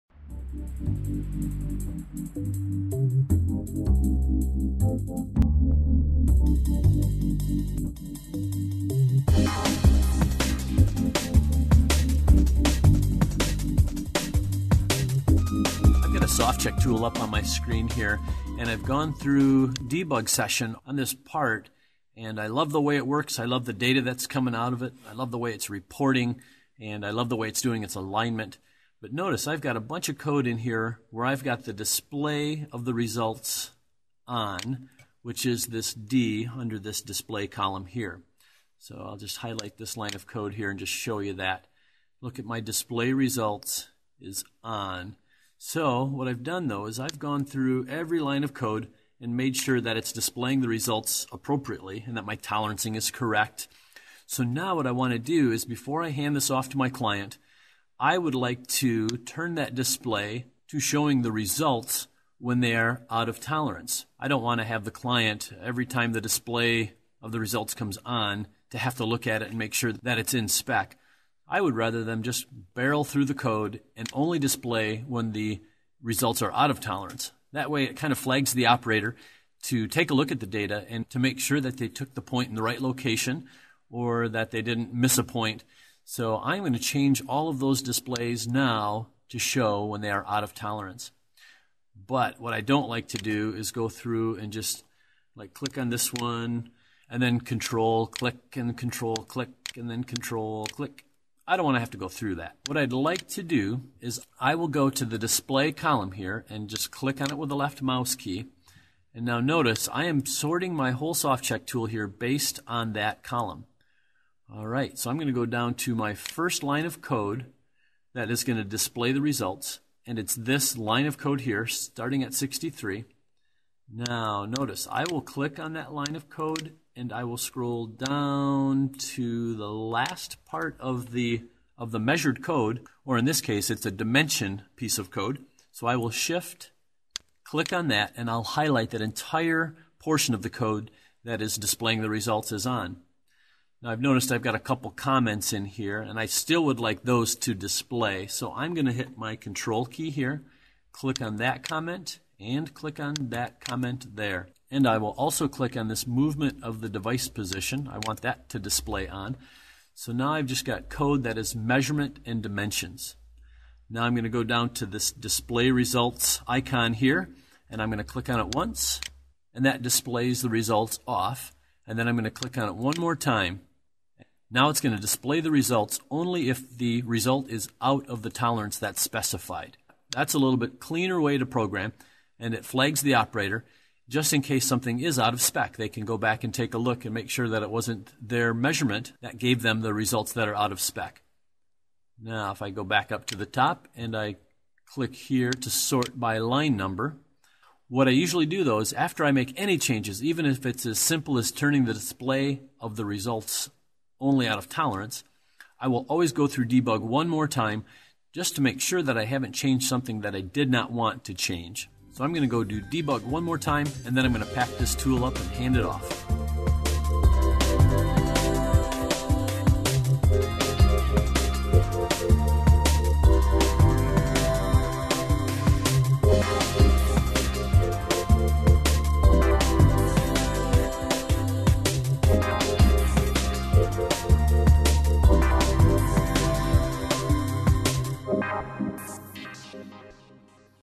Faro CAM2 Measure Tutorial: Display Results On-Off During Debug (4.0 & X)
This CAM2 Measure training video will explain how to alter your display results to show results after measurements. This is useful for debugging results in a SoftCheck.